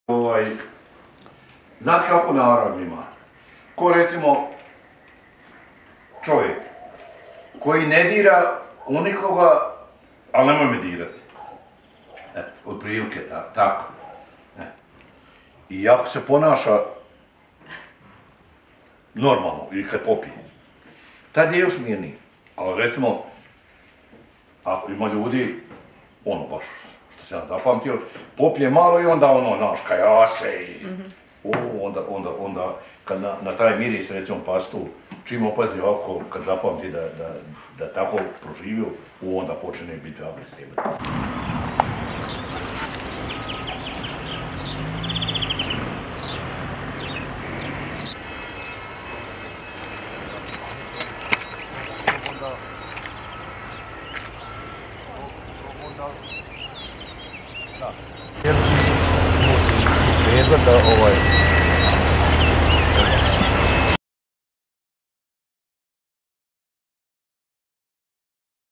Tomo Suknaić iz Majskih Poljana opisao nam je kakav je konj njegov Zekan
Posjetili smo Tomislava Suknaića i njegovog Zekana . Obitelj Suknaić u potresu koji je pogodio Banovinu ostala je bez kuće i bez škala u kojima Tomislav uzgaja konje. Uz pomoć prijatelja i donacija život obitelji Suknaić polako se vraća u normalu.